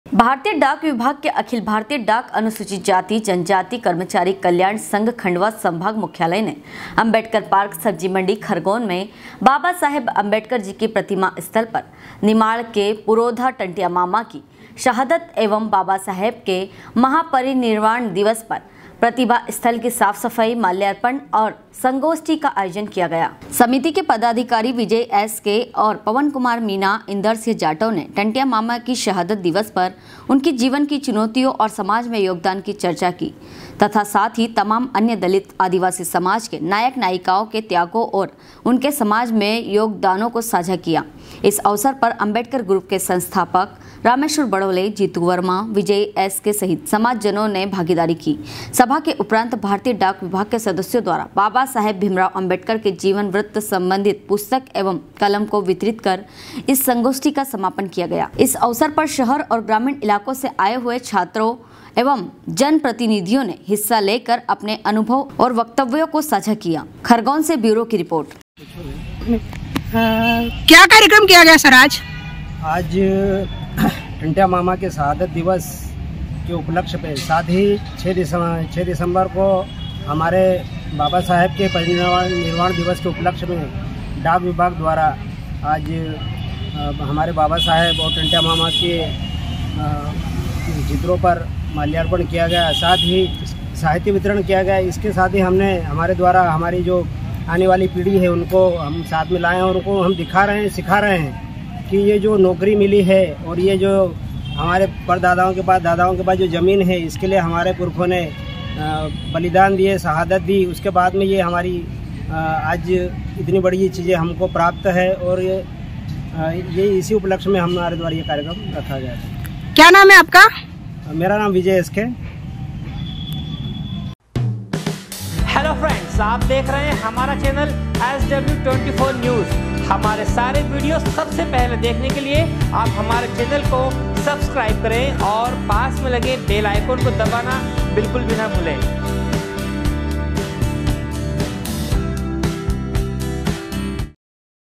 भारतीय डाक विभाग के अखिल भारतीय डाक अनुसूचित जाति/जनजाति कर्मचारी कल्याण संघ खंडवा
भारतीय डाक विभाग के अखिल भारतीय डाक अनुसूचित जाति/जनजाति कर्मचारी कल्याण संघ खंडवा संभाग मुख्यालय ने अंबेडकर पार्क सब्जी मंडी खरगोन में बाबासाहेब आंबेडकर जी प्रतिमा स्थल पर निमाड़ के पुरोधा टंट्या मामा की शहादत एवम बाबा साहेब के महापरिनिर्वाण दिवस पर प्रतिमा स्थल की साफ सफाई माल्यार्पण और संगोष्ठी का आयोजन किया जाएगा।
खरगोन से ब्यूरों की रिपोर्ट।